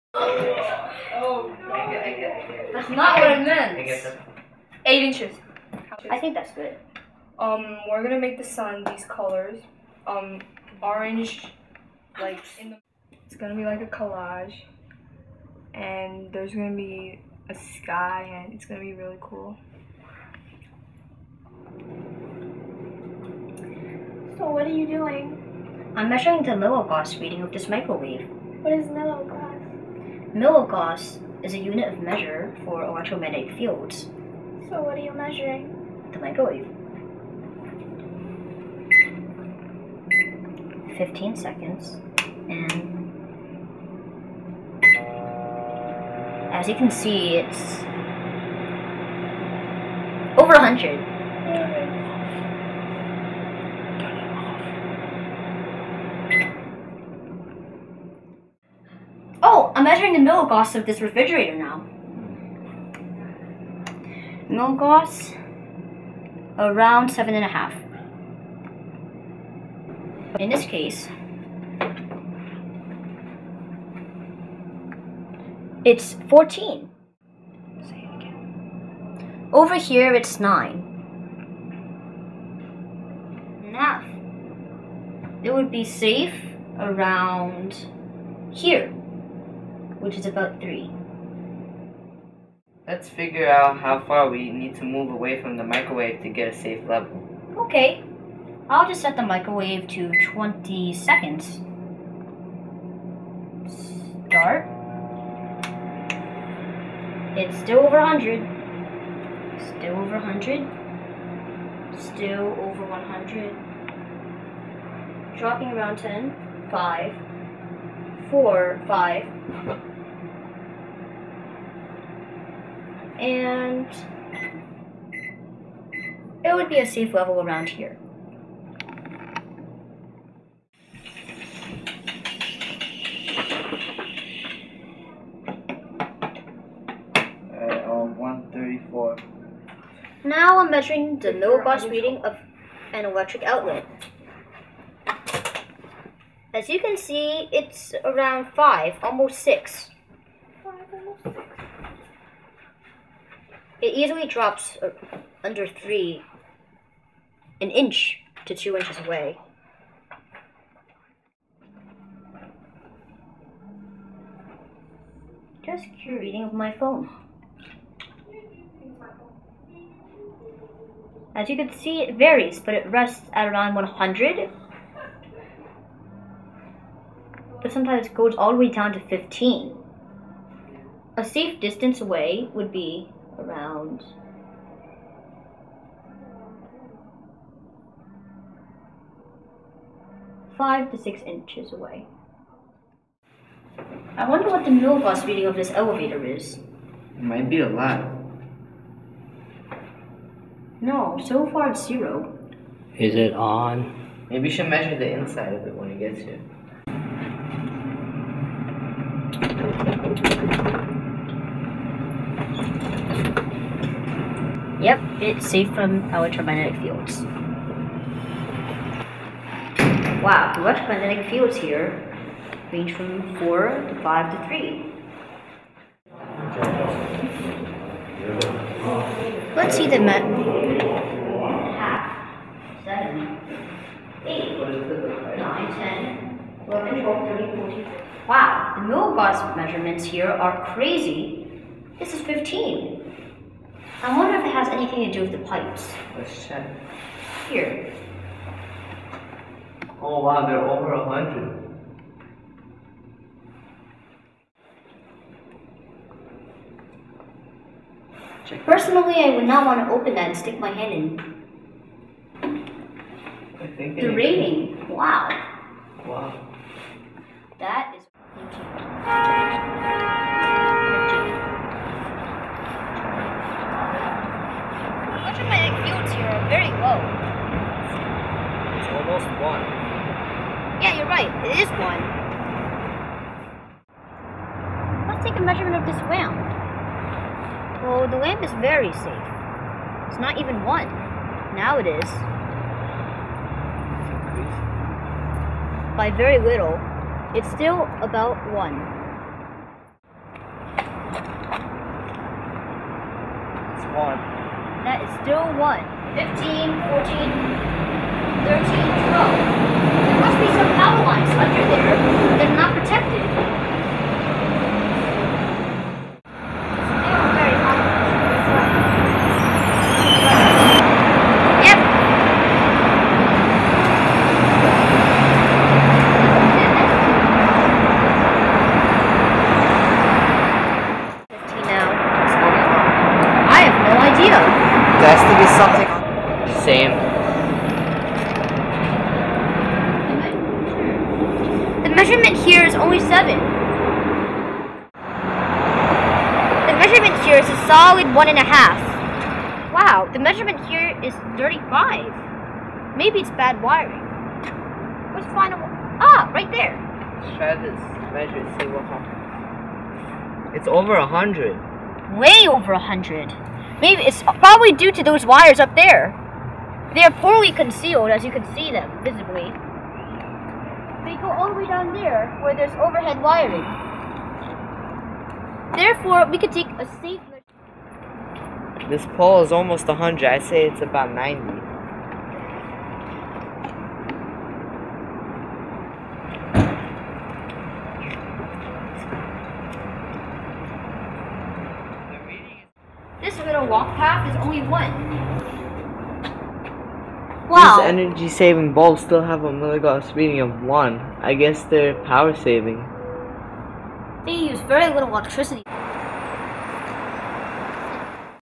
EMF Radiation in Everyday Life
Young researchers measures everyday EMF radiation around the house and on the streets of New York City
Using a TriField Meter, EMF radiation of many common devices were recorded, including an apple iphone, microwave oven, refrigerator and elevators.